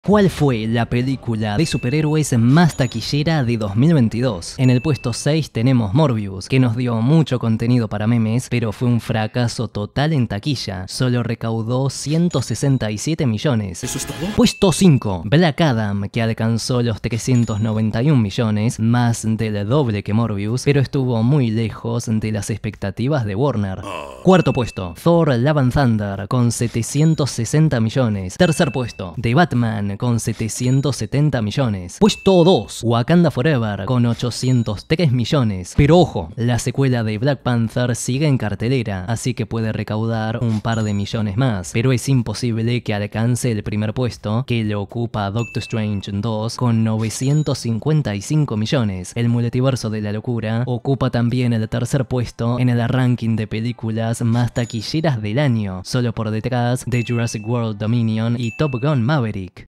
¿Cuál fue la película de superhéroes más TAQUILLERA de 2022?
¿Cuál fue la película de Marvel y DC con mayor recaudación en 2022 y por qué no fue Black Adam?